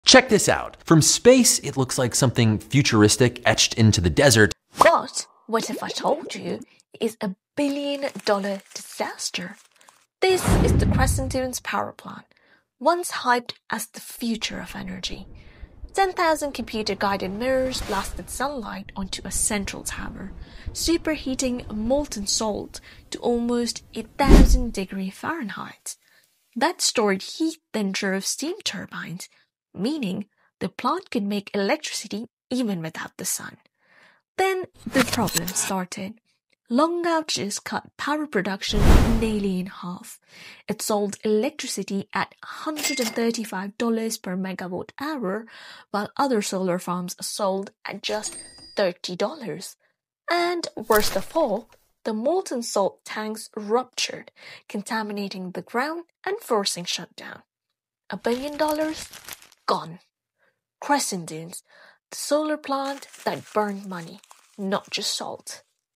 $1B Spent, 10,000 Mirrors… And It FAILED!🤯
From space, it looked like alien technology - 10,000 mirrors focusing sunlight into a single blazing point in the Nevada desert.🪞

The Crescent Dunes Solar Power Plant was a $1 billion futuristic solar power plant using molten salt heated to 1,000°F to generate electricity even after sunset.🌞☀️

But despite it was one of the most ambitious , solar energy projects that promised to redefine clean solar thermal energy production, it COLLAPSED! It was an EXPENSIVE FLOP!👎

Here’s why one of the world’s most ambitious solar megaprojects became a massive energy flop.